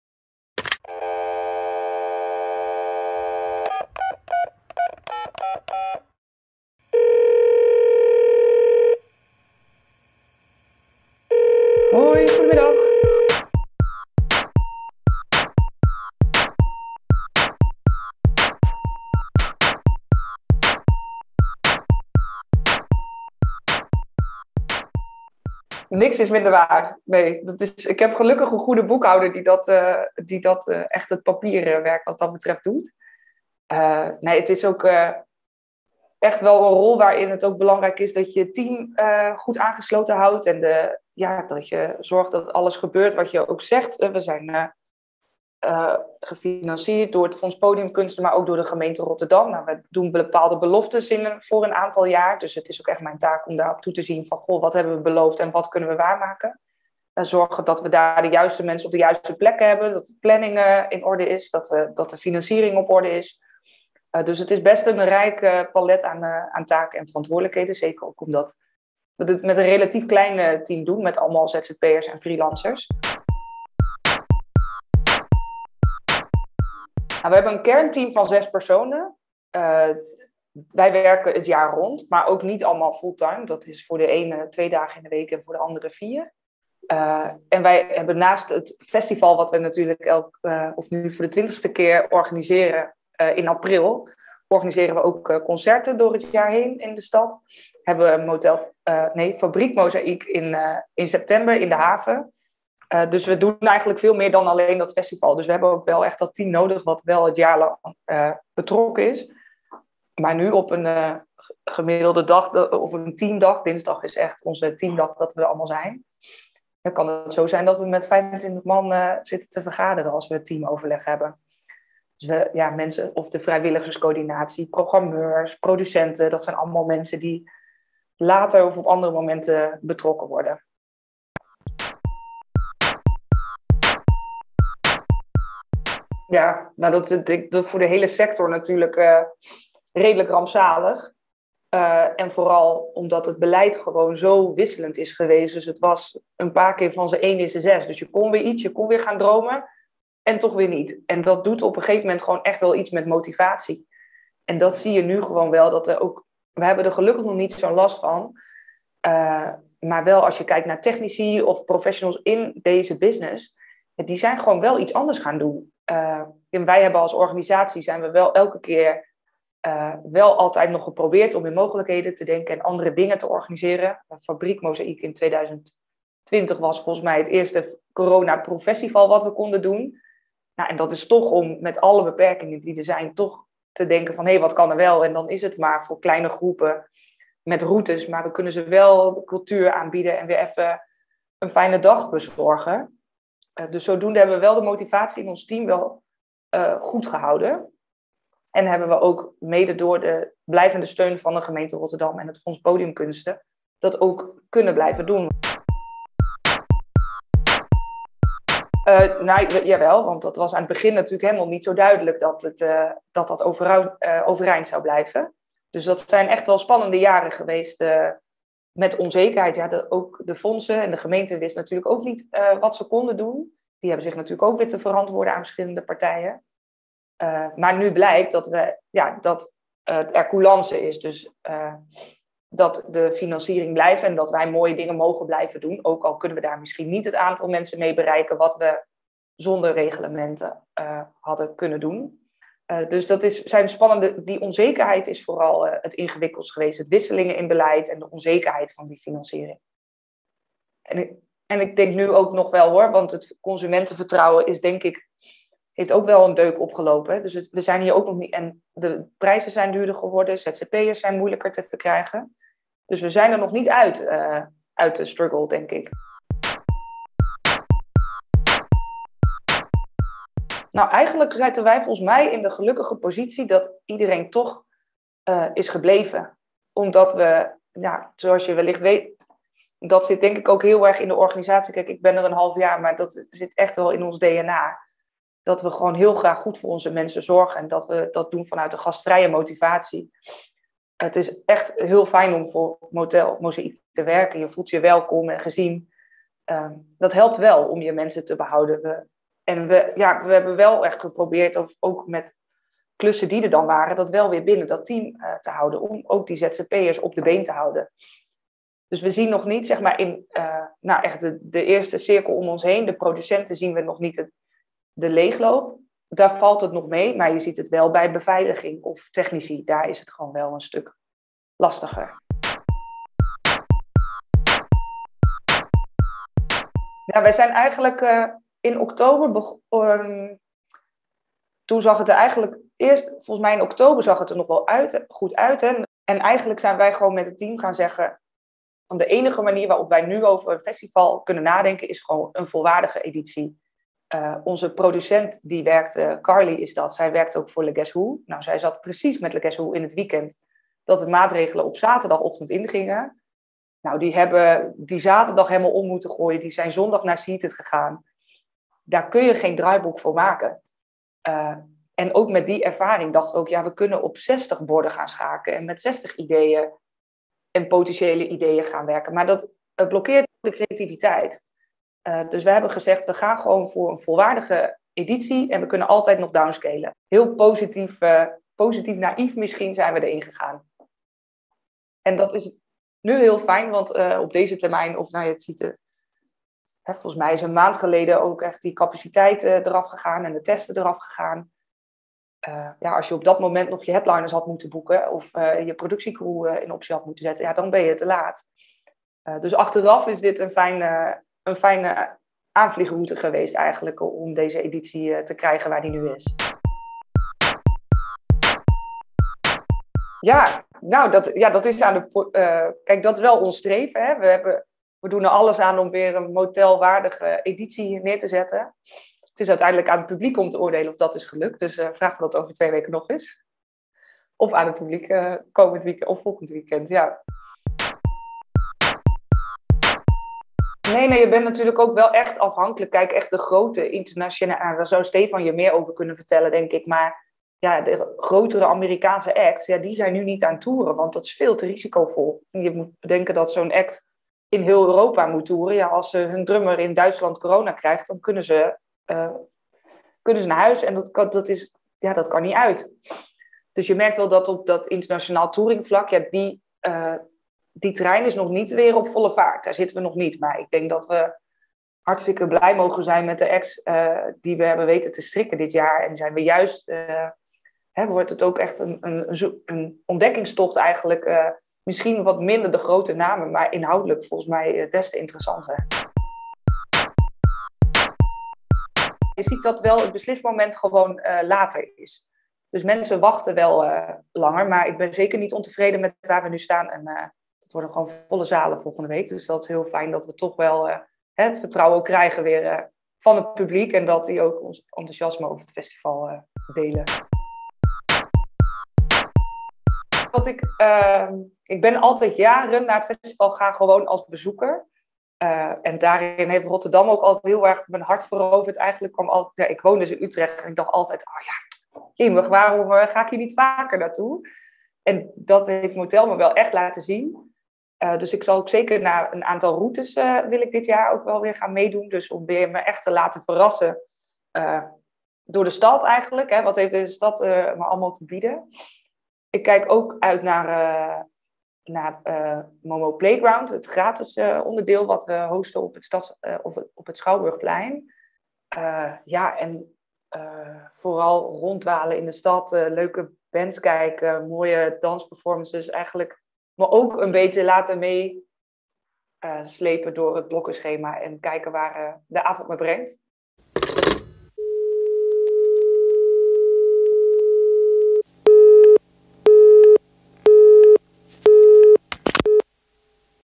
Ff bellen met Margriet Colenbrander, zakelijk directeur Motel Mozaïque
Is het voortbestaan van het festival Motel Mozaïque in gevaar geweest tijdens 2 coronajaren? Zakelkijk directeur Margriet Colenbrander geeft antwoord op deze en meer brandende vragen over Motel Mozaïque 2022.